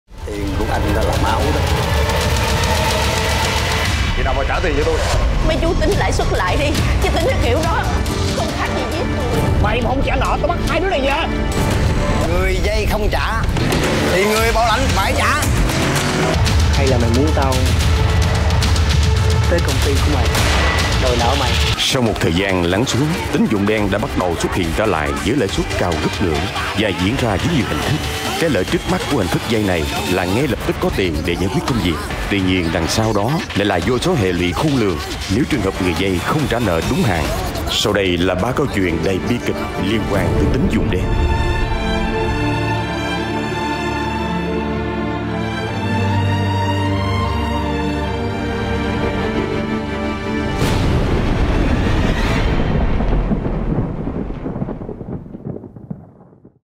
SỰ " GIÚP ĐỠ" HAY " CÁI BẪY" TỪ TÍN DỤNG ĐEN
Chỉ cần tải app" vay tiền online "mà nhiều người đã lao vào vòng xoáy nợ nần, bị các nhóm cho vay uy hiếp tinh thần lẫn tính mạng của chính mình và cả người thân. Trên đây tái hiện lại 3 trường hợp cái gọi là " Tín dụng đen"

* Your full legal name: ViVu TV